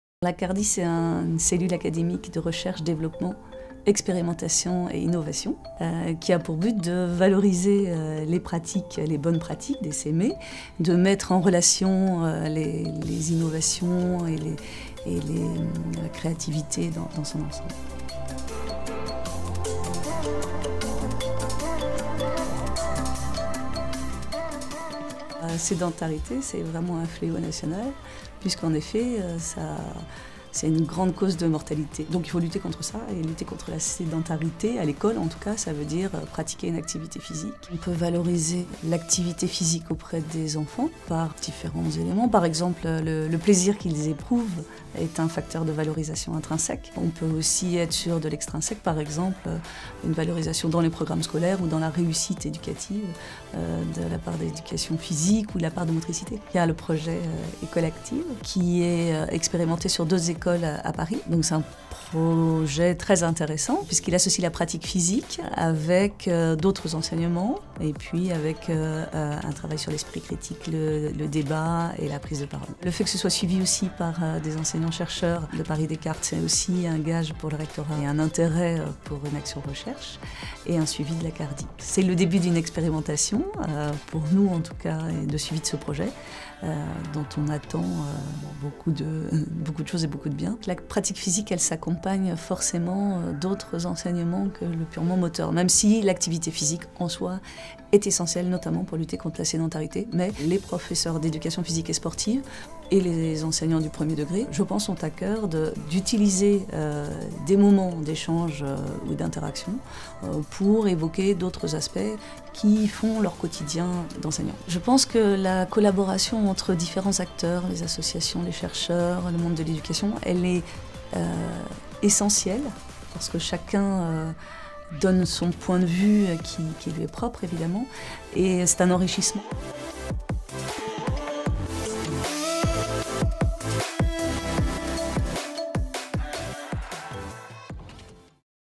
Programme Ecole Active, par Valérie Schwob. Chargée de mission Cardie, conseillère formation
TRANSMETTRE UN MODE DE VIE ACTIF !

La santé est un facteur de réussite éducative. L’école recherche continuellement à créer un environnement favorable à la santé et au bien-être des élèves.

PLAY International et l’Université Paris Descartes, département « Enjeux et Techniques du Corps » (EA3625) s’associent et mutualisent leurs outils pour construire le programme Playdagogie « École Active ». Ce curriculum s’adresse aux enseignants pour leur proposer des séances éducatives et contribuer à une culture positive des activités physiques pour la santé de leurs élèves de 6 à 12 ans.